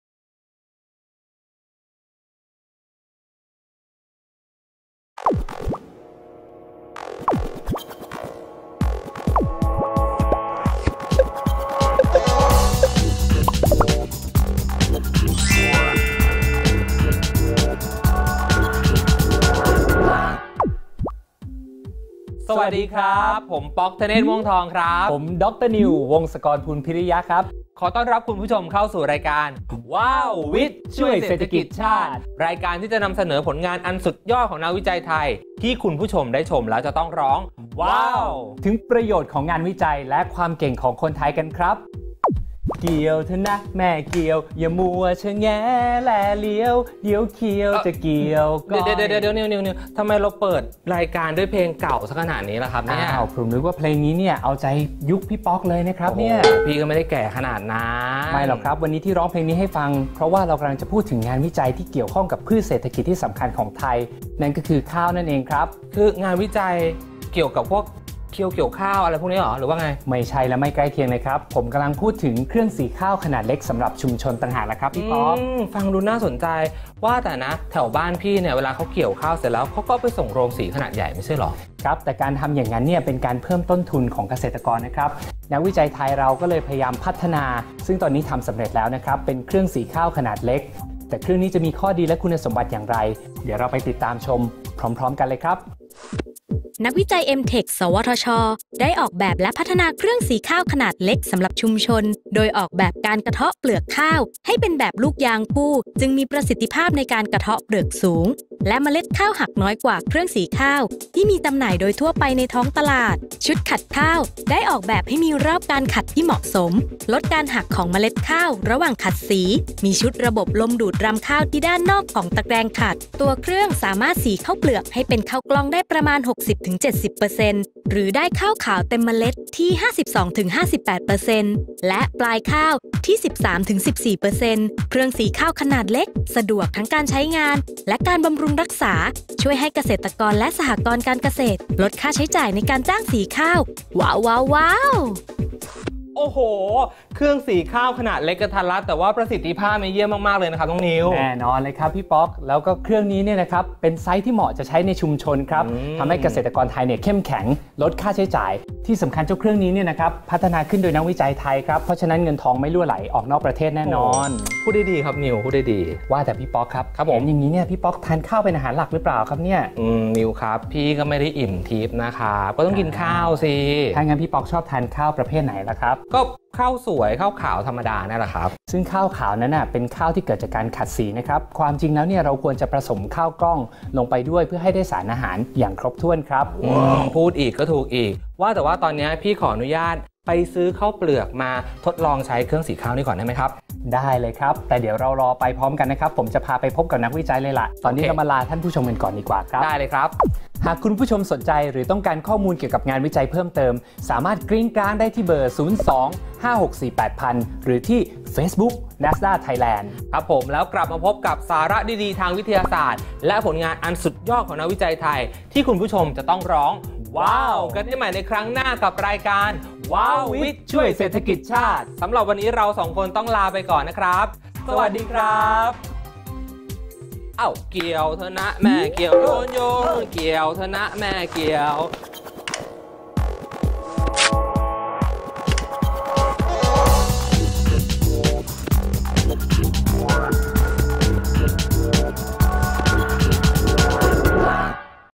ว้าววิทย์ช่วยเศรษฐกิจชาติ ตอน เครื่องสีข้าวขนาดเล็ก เพื่อชุมชนไทยเข้มแข็ง
นักวิจัย สวทช. ได้ออกแบบและพัฒนา “เครื่องสีข้าวขนาดเล็กสำหรับชุมชน” โดยออกแบบการกะเทาะเปลือกข้าว ให้เป็นแบบลูกยางคู่ จึงมีประสิทธิภาพในการกะเทาะเปลือกสูง และเมล็ดข้าวหักน้อยกว่า เครื่องสีข้าวที่มีจำหน่ายโดยทั่วไปในท้องตลาด ชุดขัดข้าว ได้ออกแบบให้มีรอบการขัดที่เหมาะสม ลดการหักของเมล็ดข้าวระหว่างขัดสี มีชุดระบบลมดูดรำข้าวที่ด้านนอกของตะแกรงขัด จึงไม่มีรำข้าวติดเมล็ดข้าวออกมา ตัวเครื่องสามารถสีข้าวเปลือกให้เป็นข้าวกล้องได้ประมาณ 60-70 % หรือได้ข้าวขาวเต็มเมล็ดที่ 52-58 % และปลายข้าวที่ 13-14 % เครื่องสีข้าวขนาดเล็กสะดวกทั้งการใช้งานและการบำรุงรักษา ช่วยให้เกษตรกรและสหกรณ์การเกษตร ลดค่าใช้จ่ายในการจ้างสีข้าวเป็นเงินหลายแสนบาทต่อปี